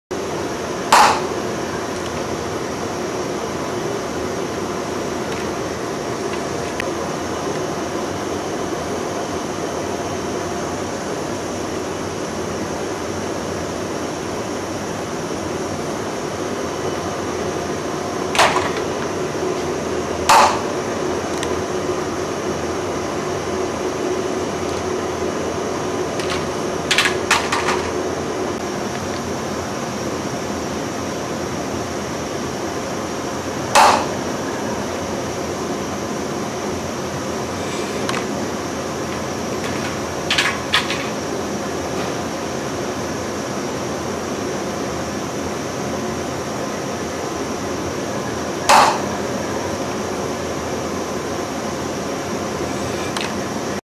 Moving head rotary cutter
Moving head rotary servo cutter to cut rigid profiles without producing any swarf.